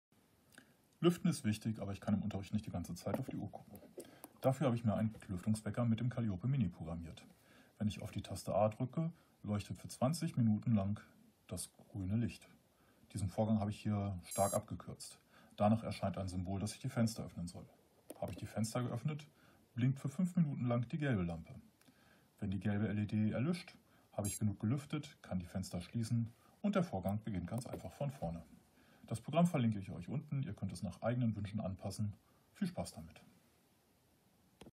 Richtig Lüften im Klassenraum mit dem Lüftungswecker - Calliope mini
Ich zeige euch hier einen praktischen Lüftungswecker mit dem Calliope mini.

Dieser passt auf, dass wir die 20-5-20 Lüftungsregeln einhalten, ohne im Unterricht dauernd auf die Uhr zu schauen zu müssen.
Den Quellcode und das Blockly Bild aus dem Open Roberta Editor habe ich euch hier verlinkt

Der Code kann nach belieben individuell angepasst werden.